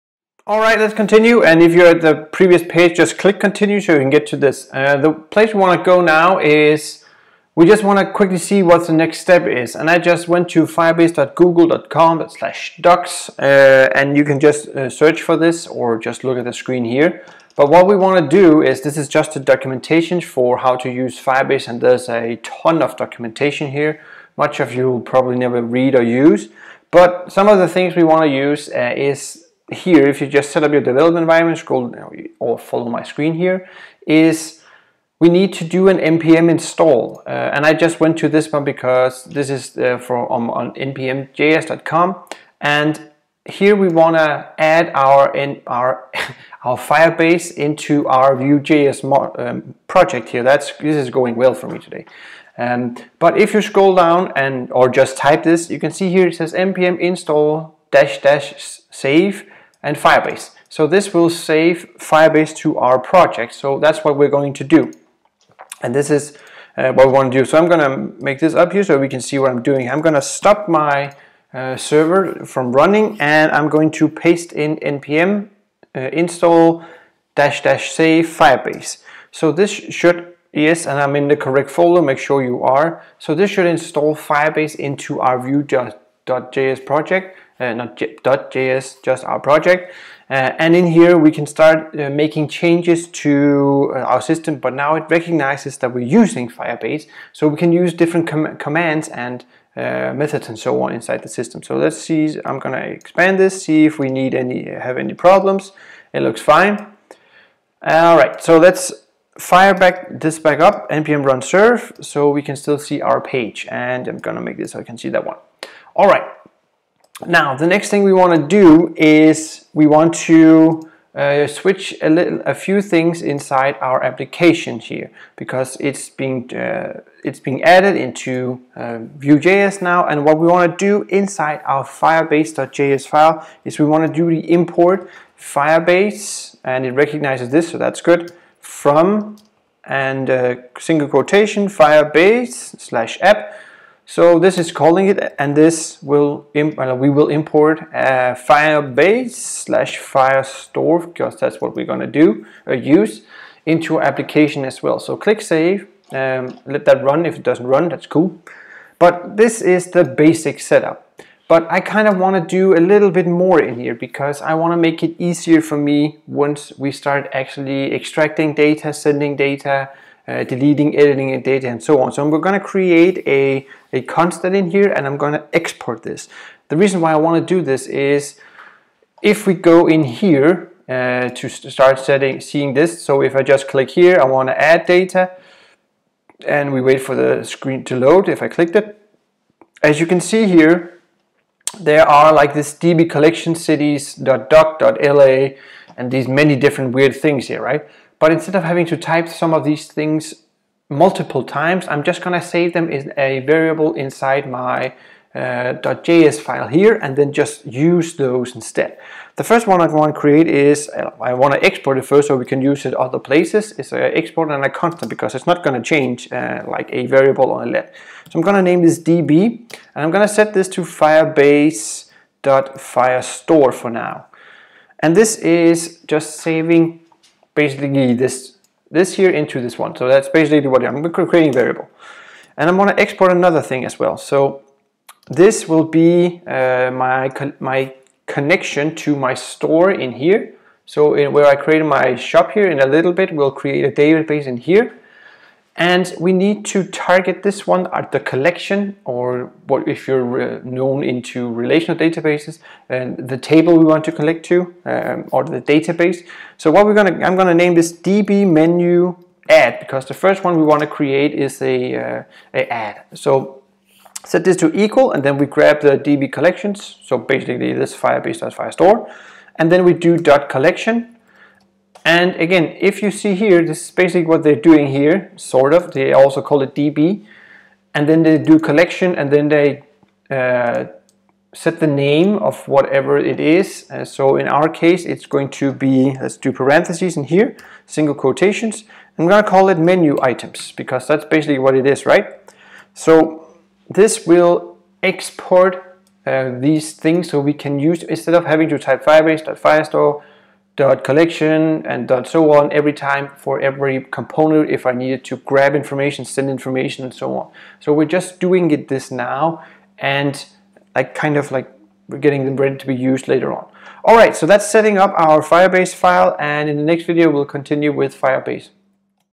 VueJS | Lvl 5 25 npm install firebase + firebase js setup (check extra video 25.0)
Missing how to create firebase.js file - Will make a bonus video to show that + a few other things